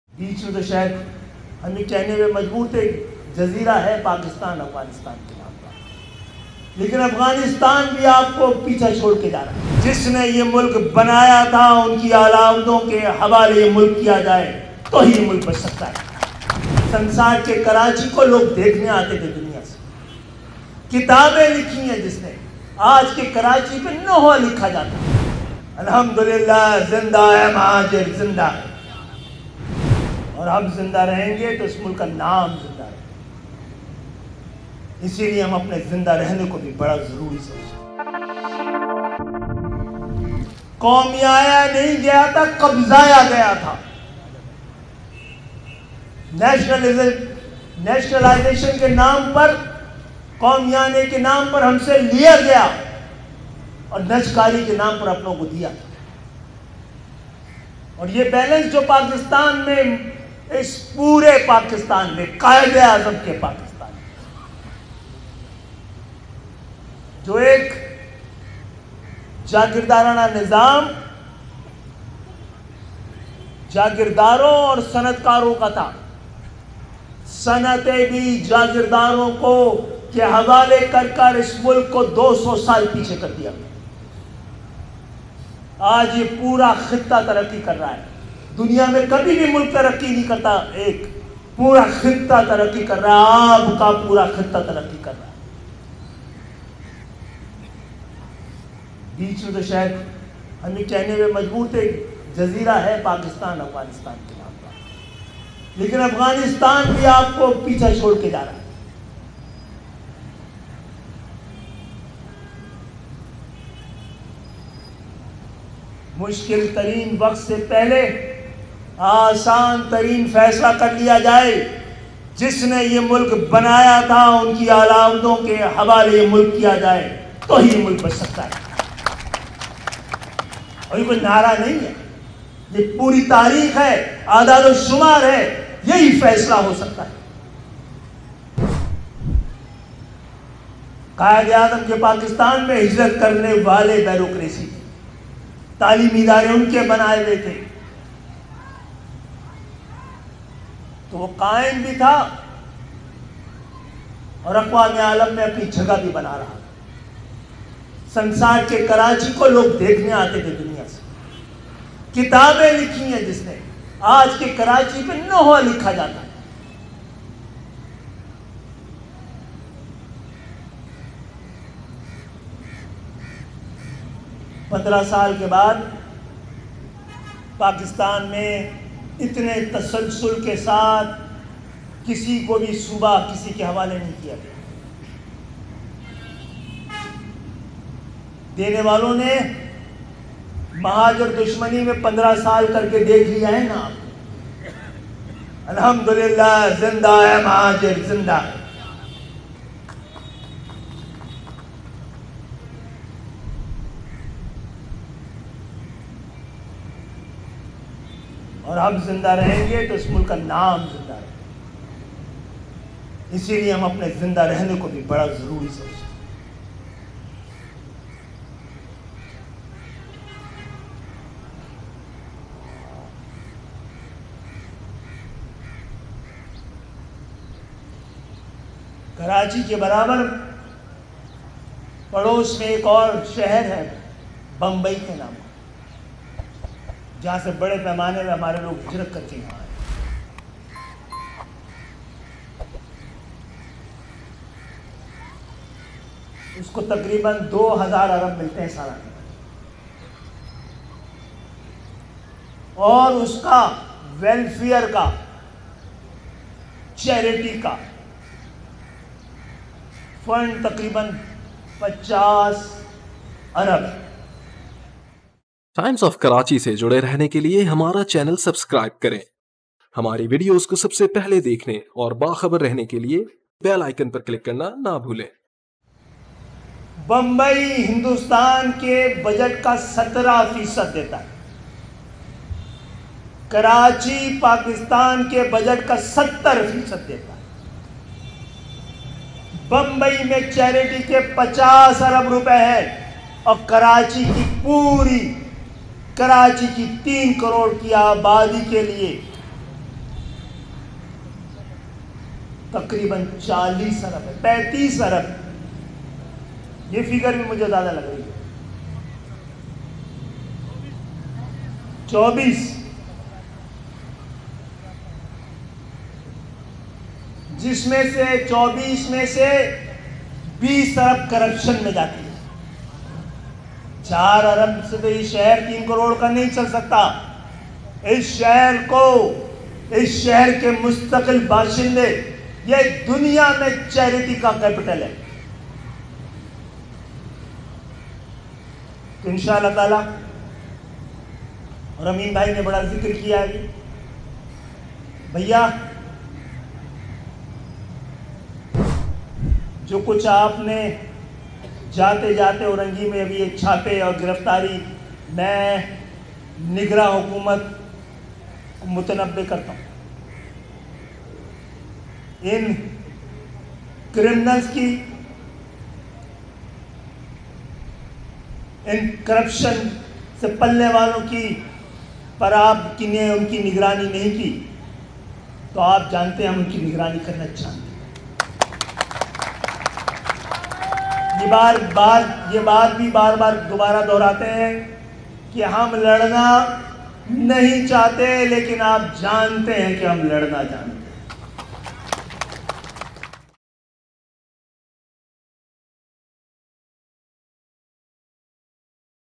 "Pehlay Karachi Per Kitab Likhi Jati Thi, Aj Noha Likha Jata Hai", Khalid Maqbool Siddiqui
"Pehlay Karachi Per Kitab Likhi Jati Thi, Aj Noha Likha Jata Hai", said Khalid Maqbool Siddiqui.